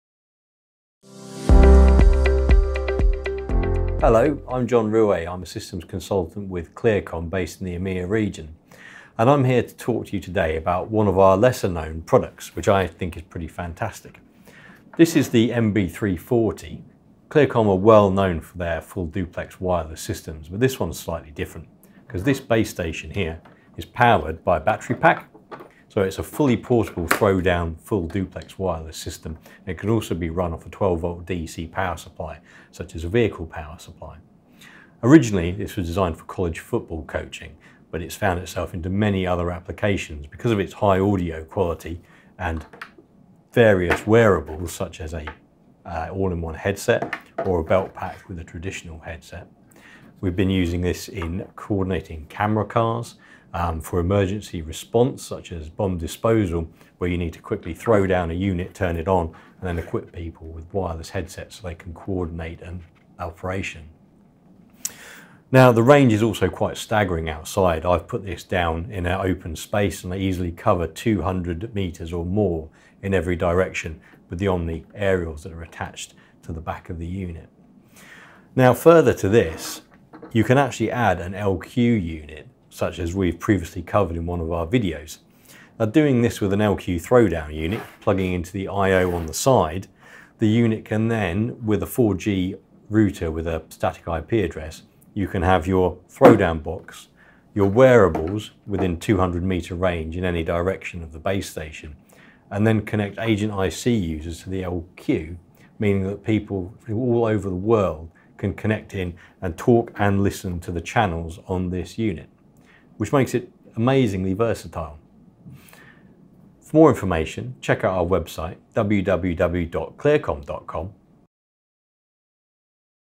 DX340ES Portable Duplex Wireless Overview
This week on Just Johns Comms, John R. looks at the portable DX wireless base and its applications and the versatility of adding in remote and IP  connectivity through LQ Series & Agent-IC.